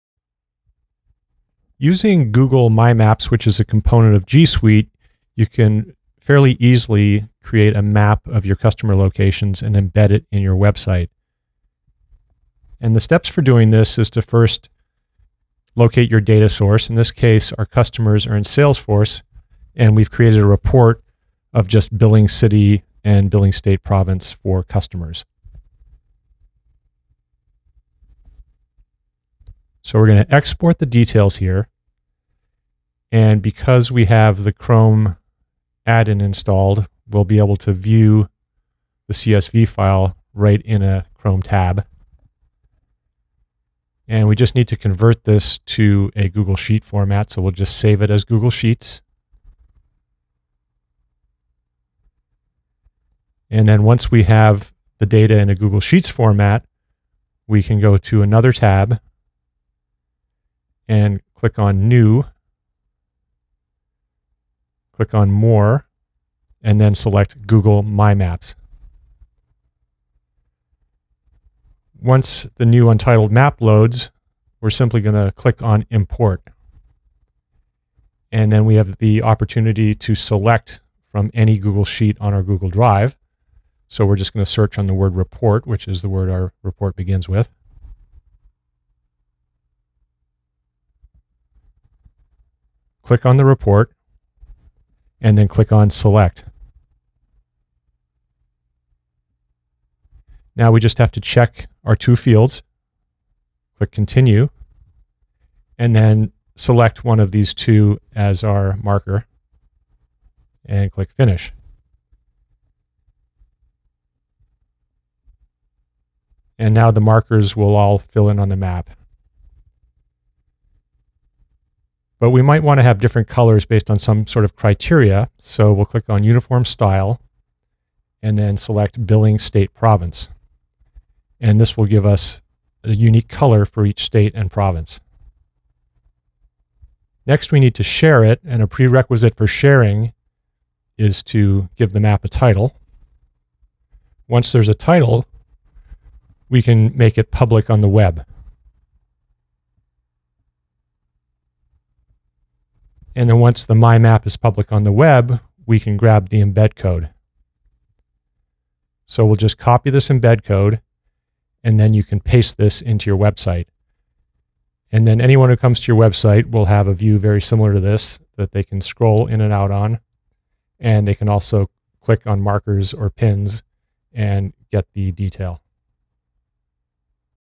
Google My Maps: Creating a Map in Personal Gmail or in Business G Suite
Step by step instructions on how to add a my map of customer locations (city and state/province) to your website.

12 Reasons to Move Your Business From Free Gmail to G Suite